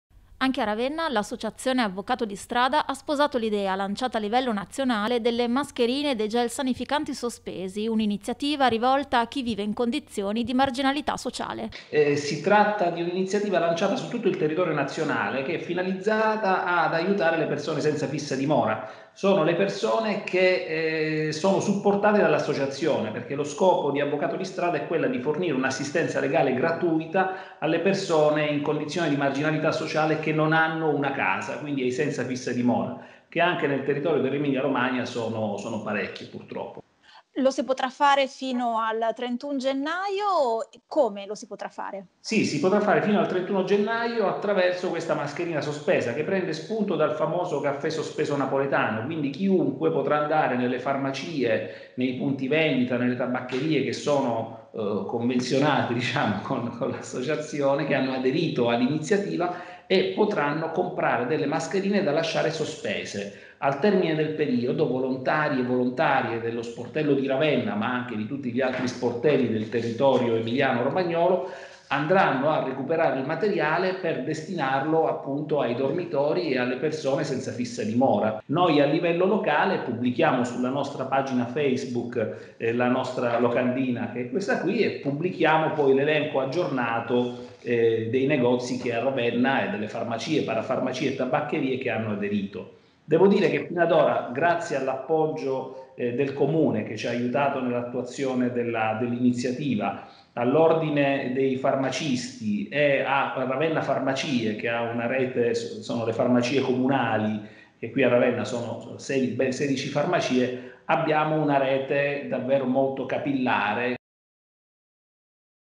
Ravenna, mascherine e gel sanificante “sospesi” grazie ad “Avvocato di strada”
Anche a Ravenna l’associazione Avvocato di Strada ha sposato l’idea, lanciata a livello nazionale, delle mascherine e dei gel sanificanti “sospesi”, un’iniziativa rivolta a chi vive in condizioni di marginalità sociale.